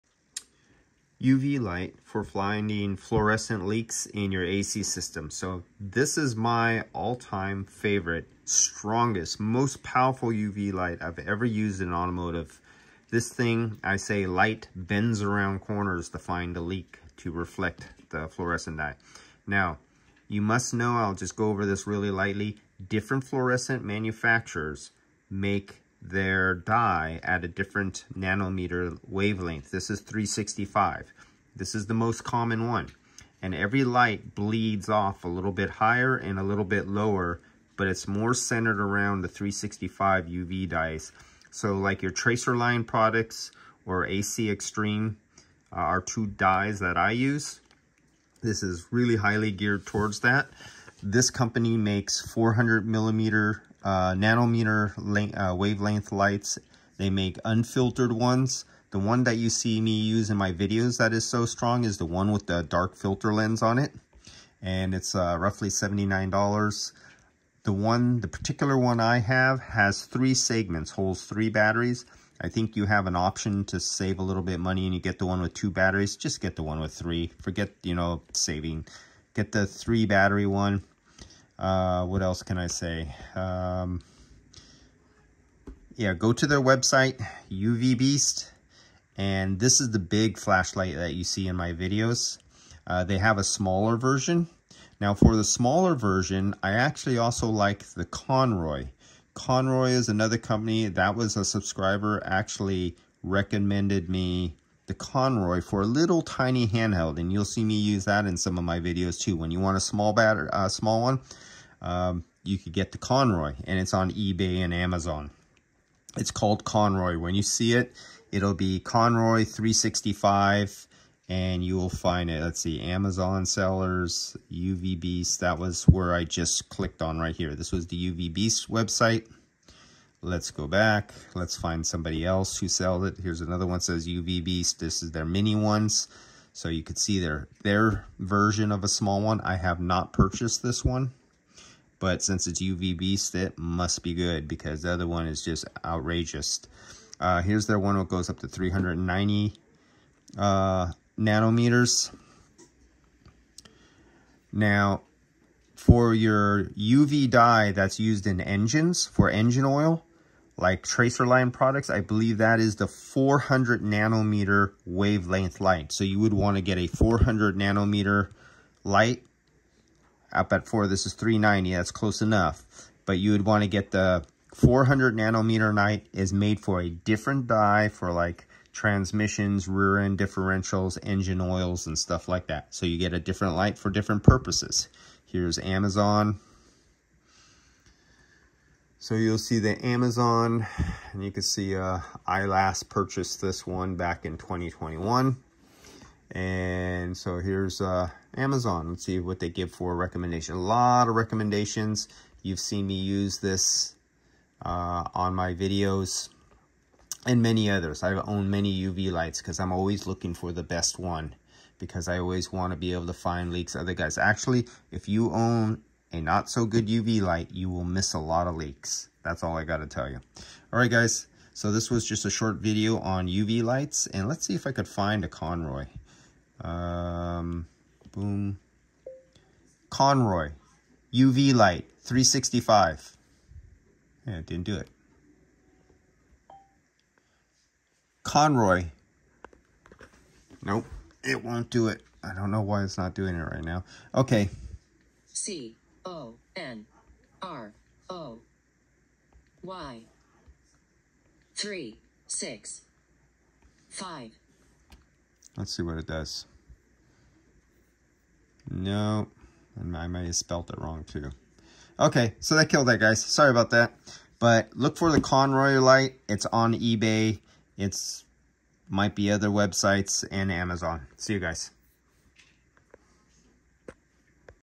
uvBEAST BLACK LIGHT V3 365nm this is the best UV light I have ever found for automotive leak test
Automotive air-conditioning, UV light leak detection